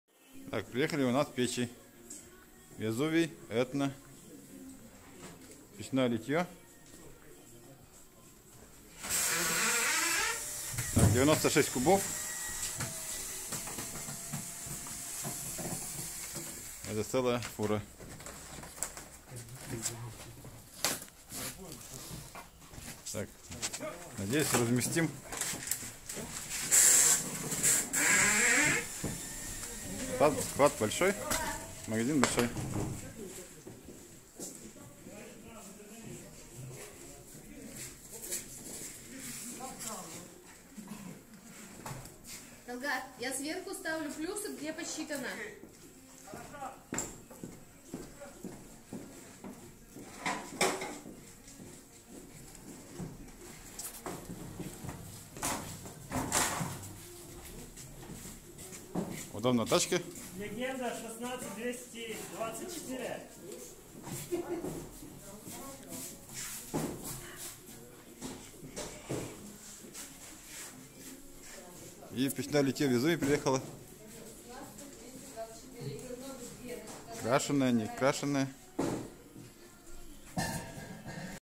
Печное и каминное литье Везувий . Чугунные банные печи "Легенда".
Новое поступление печного и каминного литья, банные печи, камины и котлы ТМ "Везувий" в магазин "Печар" г. Киров ул. Маклина, 77